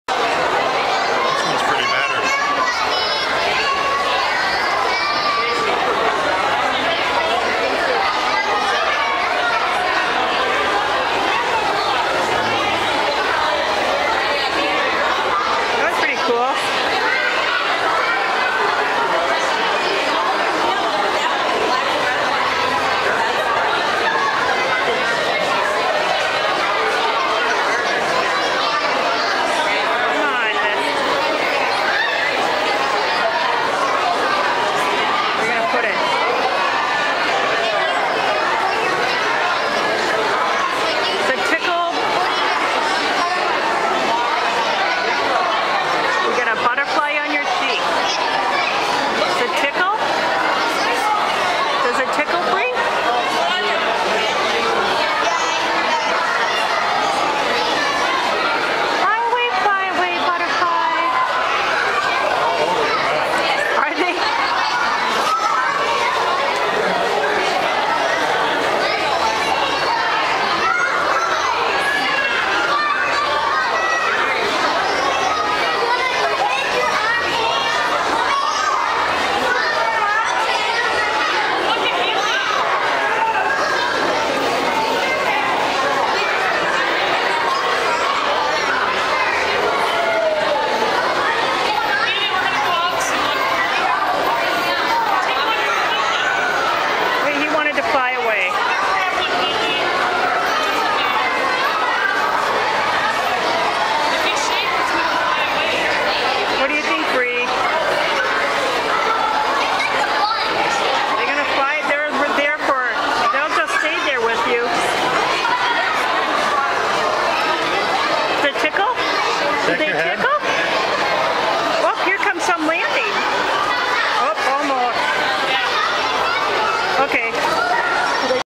BREANNA in the Butterfly Tent
Breanna Thingvall in the Minnesota State Fair Butterfly Tent